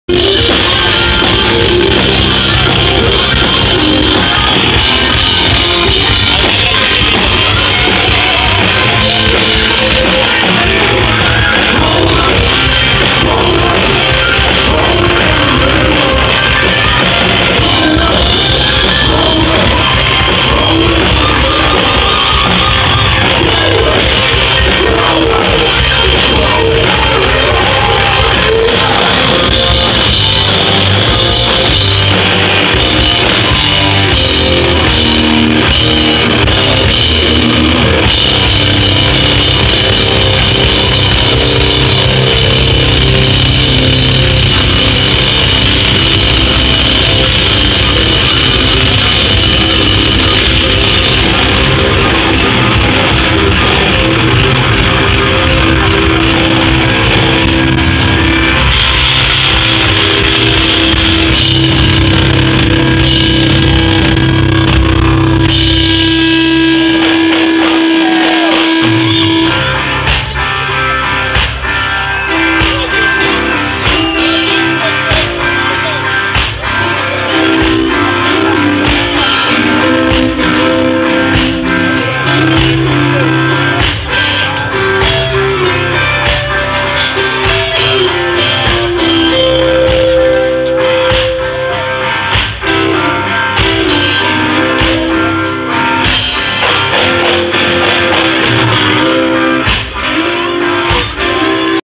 ŻARY ACID DRINKERS JUWENALIA 2008
KONCERT ACID DRINKERS POD RATUSZEM W ZARACH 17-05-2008